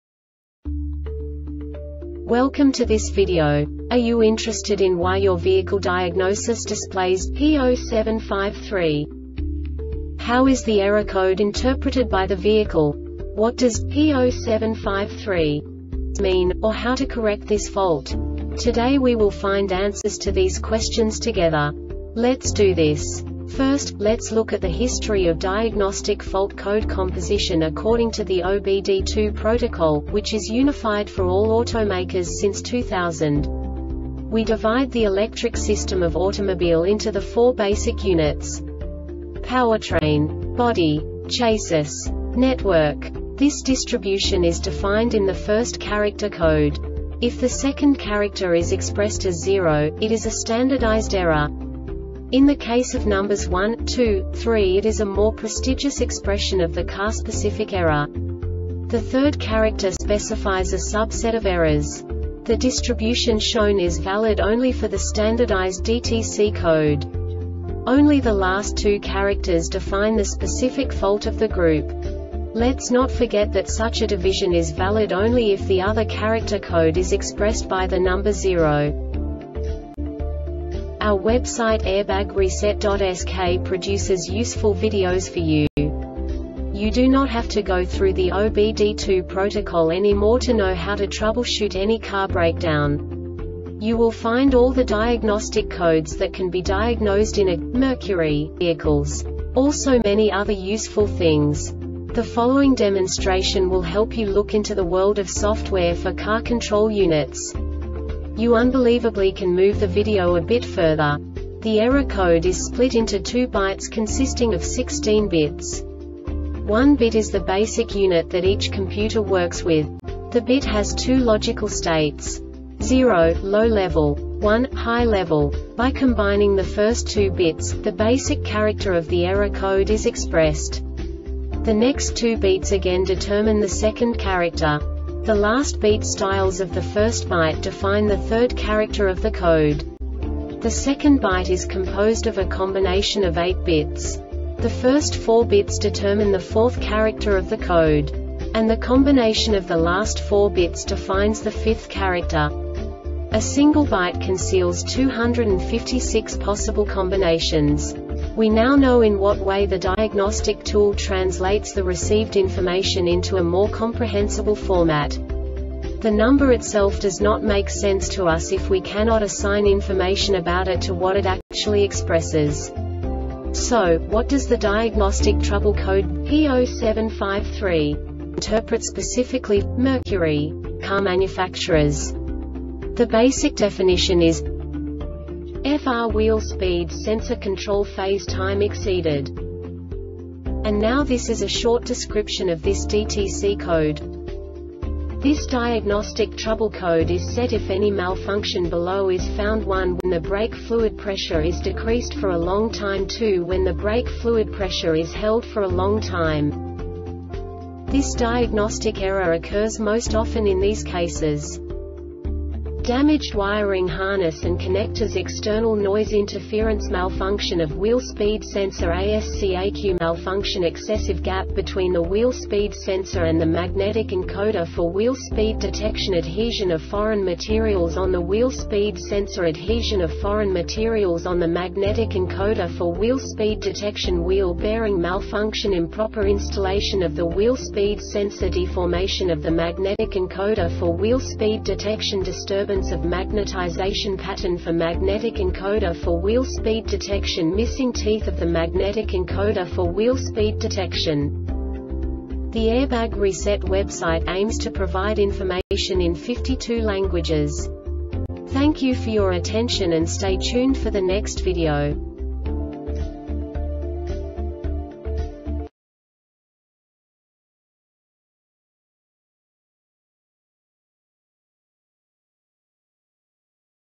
DTC Mercury P0753 Short Explanation
Contents:
0:21 Basic DTC analysis according to OBD2 protocol standard.
1:48 Insight into programming
2:58 A diagnostically understandable description of the Diagnostic Trouble Code
3:05 Basic error definition

Make:
Mercury
Code:
P0753
Definition:
FR wheel speed sensor control phase time exceeded
Description:
This diagnostic trouble code is set if any malfunction below is found: 1).When the brake fluid pressure is decreased for a long time.2).When the brake fluid pressure is held for a long time.